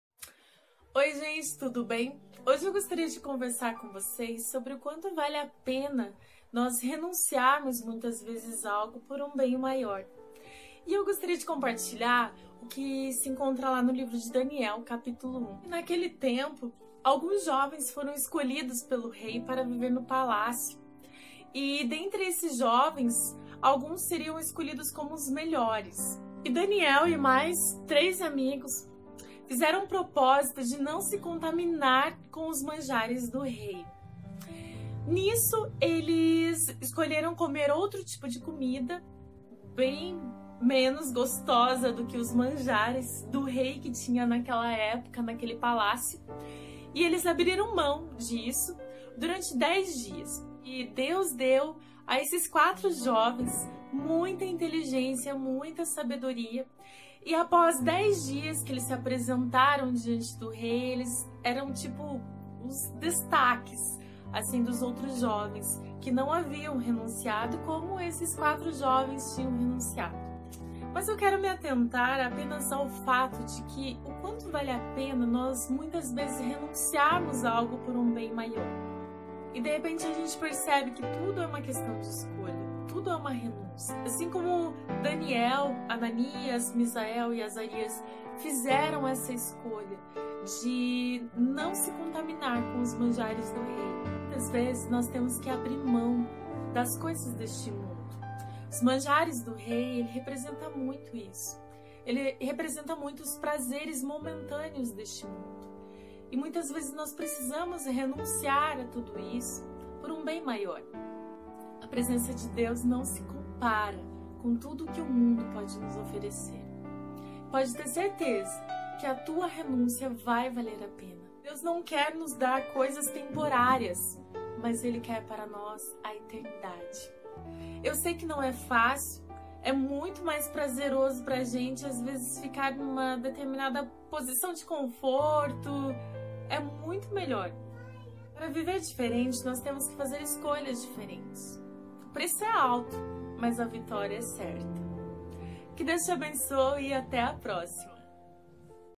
O Poder da RENÚNCIA - Devocional
Devocional sobre o poder de renúncia algo por um bem maior. Os detalhes dessa história você encontra na Bíblia, no livro de Daniel, capítulo 1.

Devocional, louvor, gospel, mensagem, palavra, Deus, fé, edificação, motivação, motivacional,